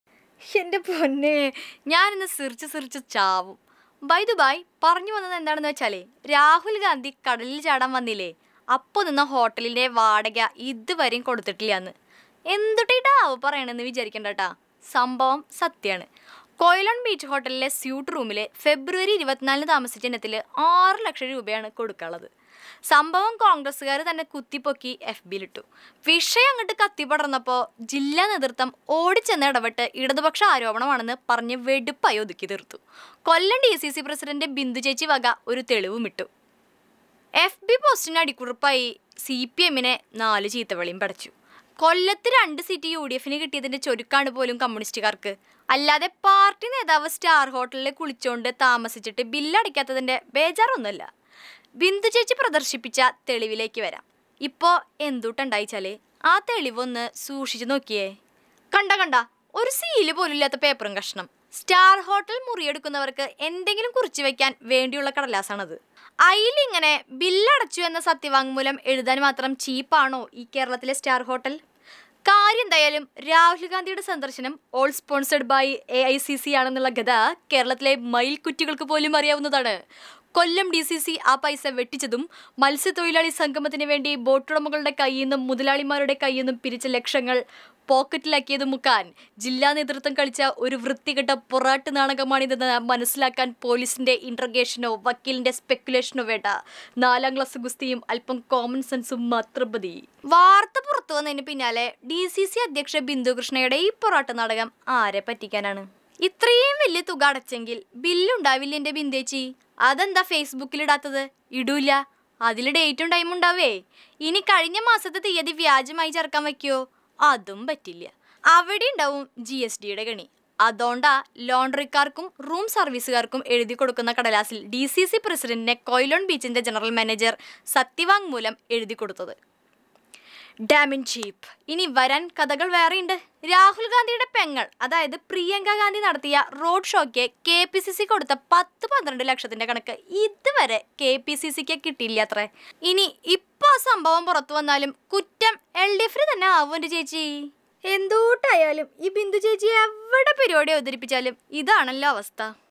ബിന്ദു ചേച്ചി എവിടെ പരിപാടി അവതരിപ്പിച്ചാലും ഇതാണല്ലോ അവസ്ഥ? | Rahul gandhi | Bindhu Krishna
ഇന്റെ പൊന്നോ ഞാനിന്ന് സിറിച്ച് സിറിച്ച് ചാവും. ബൈ ദുബായ്. പറഞ്ഞു വന്നതെന്താണെന്നു വച്ചാലേ.. രാഹുൽ ​ഗാന്ധി കടലിൽ ചാടാൻ വന്നീലെ അപ്പോ നിന്ന ഹോട്ടലിലെ വാടക ഇതുവരേം കൊടുത്തില്ലാന്ന്. എന്തൂട്ടാ ഈ ഡാവി പറേണേന്ന് വിചാരിക്കണ്ടാ ട്ടാ.. സംഭവം സത്യാണ് കൊയ്‌ലോൺ ബീച്ച് ഹോട്ടലിലെ സ്യൂട്ട് റൂമില് ഫെബ്രുവരി 24 ന് താമസിച്ച ഇനത്തില് 6 ലക്ഷം രൂപയാണ് കൊടുക്കാനുള്ളത്. സംഭവം കോൺഗ്രസ്സുകാർ തന്നെ കുത്തിപ്പൊക്കി എഫ്‌ബിലിട്ടു. വിഷയങ്ങട് കത്തിപ്പടർന്നപ്പോ ജില്ലാ നേതൃത്വം ഓടിച്ചെന്നു ഇടപെട്ട് ഇടതുപക്ഷ ആരോപണമാണെന്ന് പറഞ്ഞ് വെടിപ്പായി ഒതുക്കി തീർത്തു.